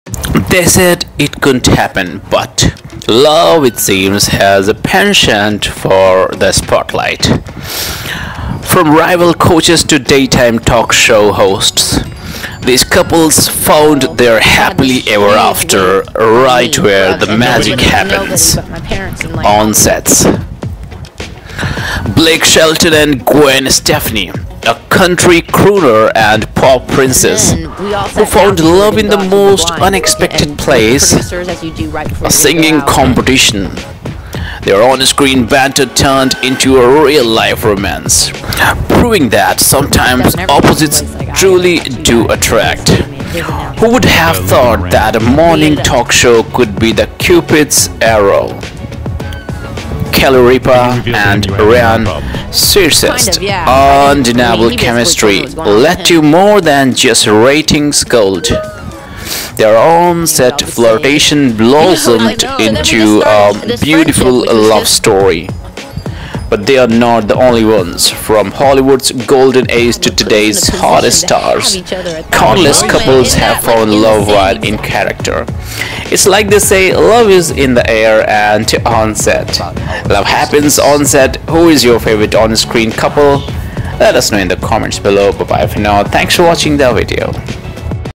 Blake Shelton And Gwen Stefani Meeting Each Other Post-breakup To Kelly Ripa Slipping The 😲
celebrityfactsnsecrets From Blake Shelton and Gwen Stefani meeting each other post-breakup to Kelly Ripa slipping the, these couples fell in love on set.